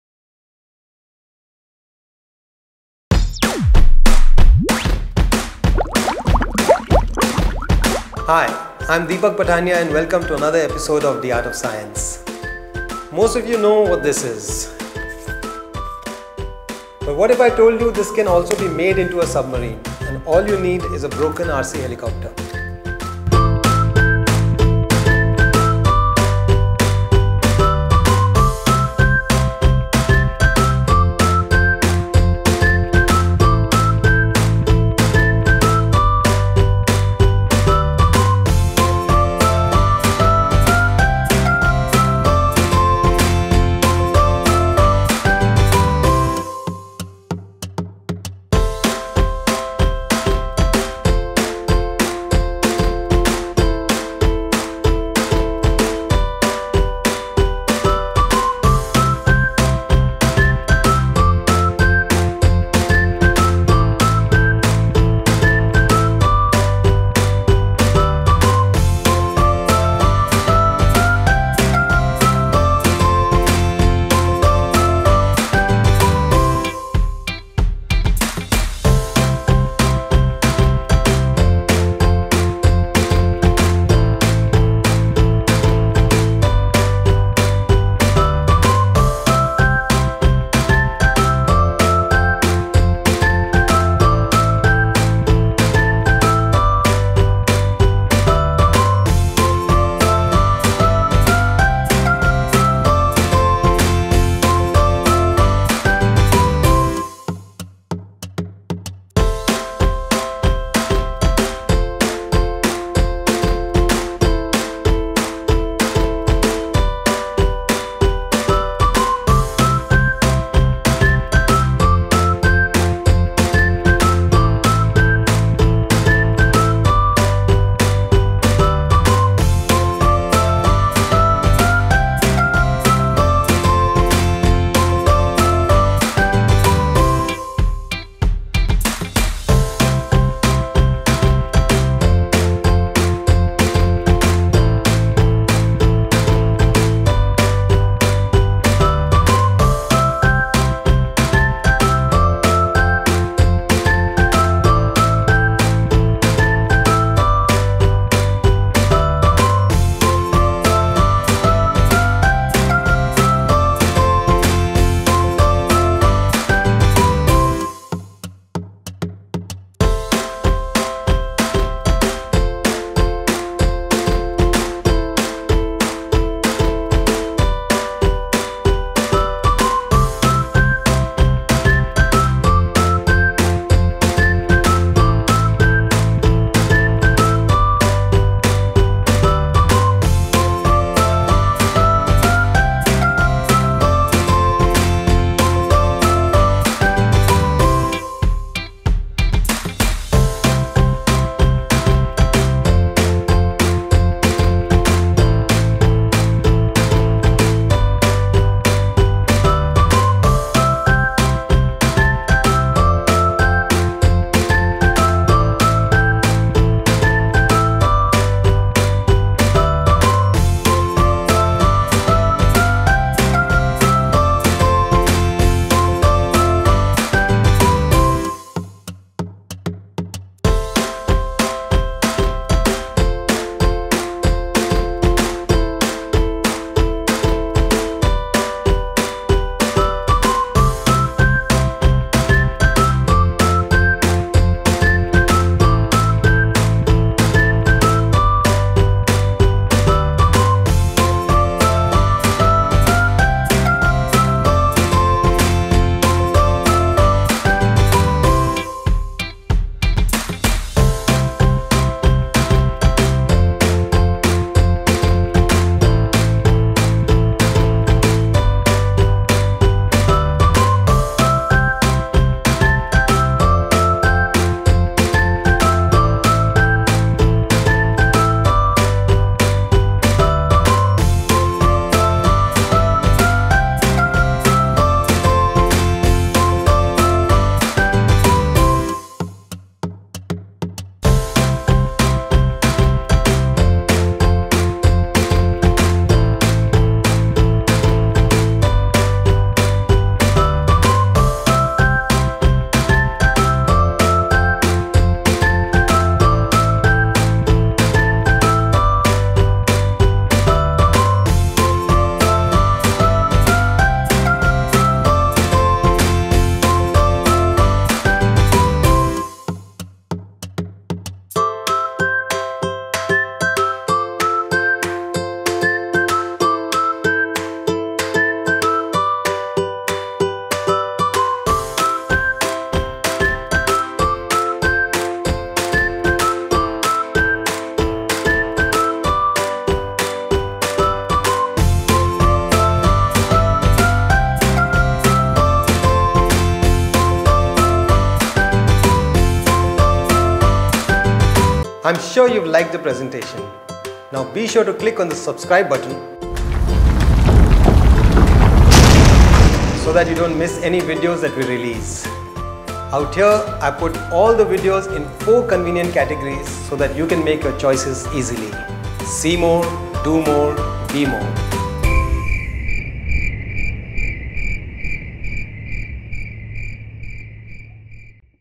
How to make a RC Submarine? | DIY RC Submarine | DArtofScience
See how to make a very simple DIY submarine using a broken Remote Controlled Helicopter.
This DIY RC Submarine experiment is a very simple project for kids.

Submarines go up and down in the water by changing its weight by displacing water...but how do humans go deeper or come up to the surface? We propel ourselves in the down or up direction using our hands and legs. in this video we make use of a similar technique of using a propellor to dive deeper or float to the top and have made probably the worlds quickest DIY RC submarine.

Without the lights then one can actually make this under 5-10 mins.